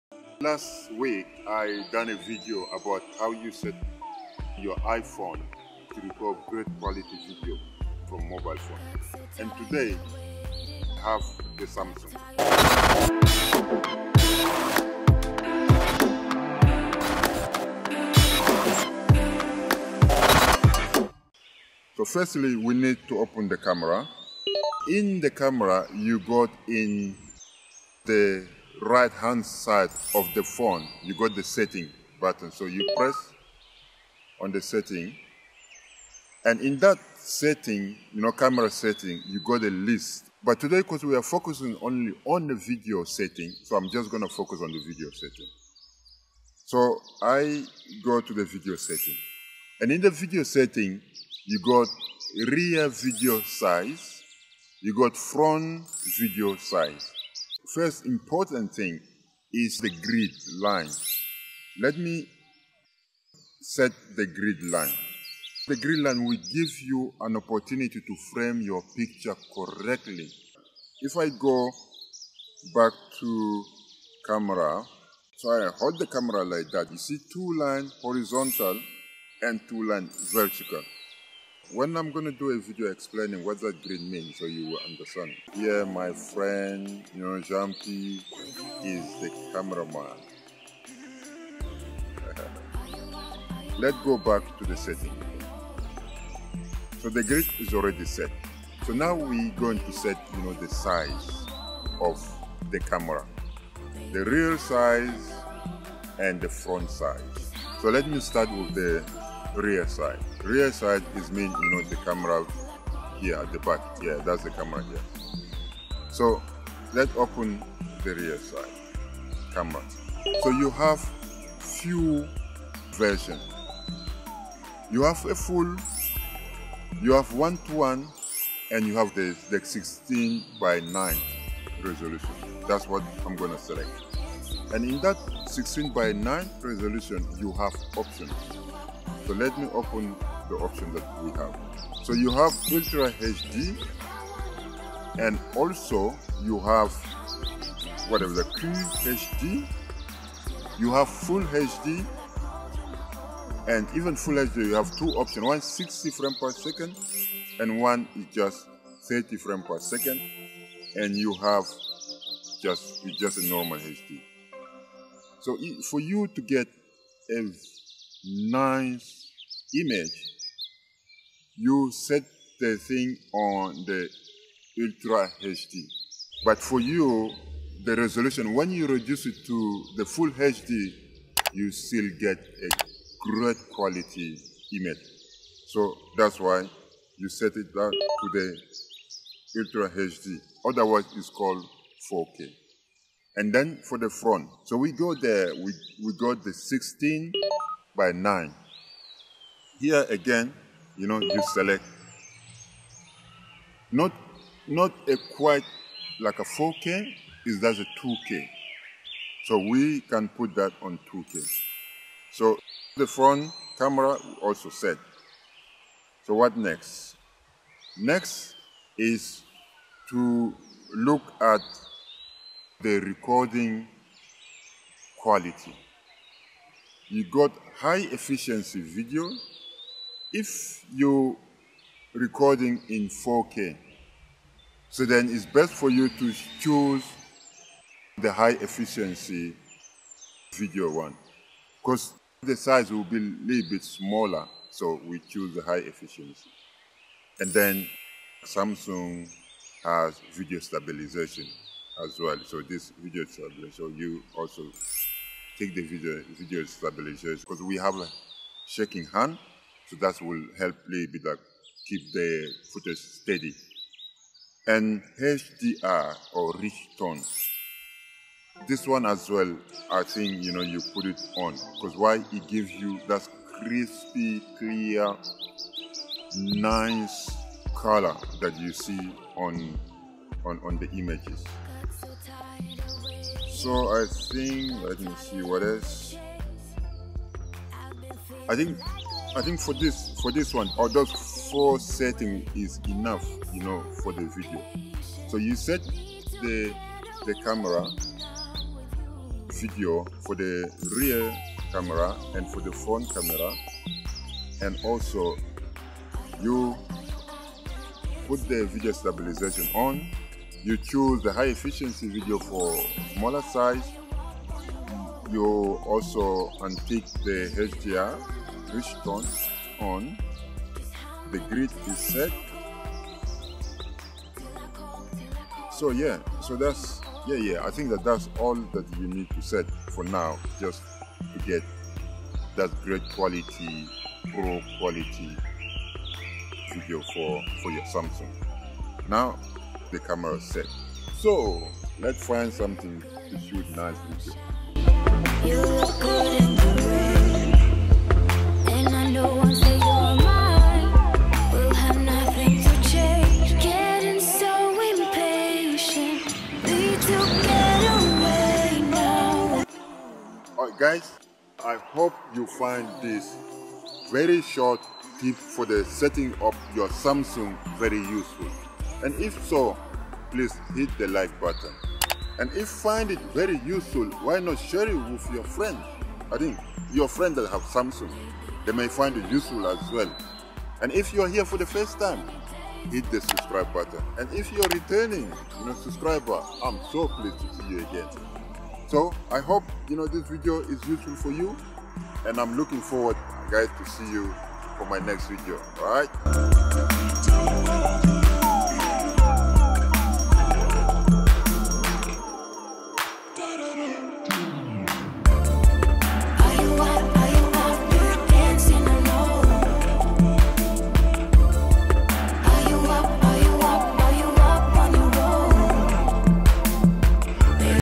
Set Samsung Galaxy to get better videos
Your Samsung is hiding with great features to help Capture great quality video.

With this video, you will learn how to set up.

You just need to set your camera there is so much potential for recording videos.